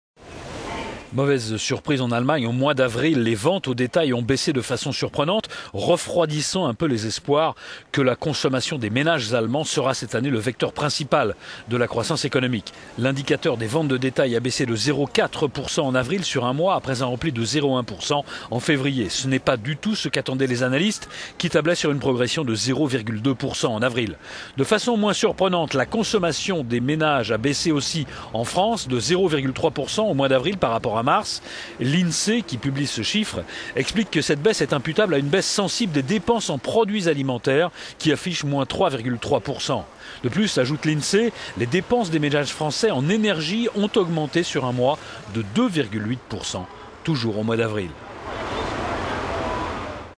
Baisse des dépenses de consommation en France et en Allemagne en avril - economy
Mauvaise surprise en Allemagne : au mois d'avril, les ventes au détail ont baissé de façon surprenante, refroidissant un peu les espoirs que la consommation des ménages allemands sera cette année le vecteur principal de la croissance économique.
L'indicateur des ventes de détail a baissé de 0,4% en avril sur un mois après un repli de 0,1% en février.
ça n'est pas du tout ce qu'attendaient les analystes qui tablaient sur une progression de 0,2% en avril.
De façon moins surprenante, la consommation des ménages a baissé aussi en France de 0,3% au mois d'avril par rapport à mars. L'INSEE, l'institut national de la statistique qui publie ce chiffre explique que cette baisse est imputable à une baisse sensible des dépenses en produits alimentaires qui affiche -3,3%. De plus ajoute l'INSEE, les dépenses des ménages français en énergie ont augmenté sur un mois de 2,8% en avril.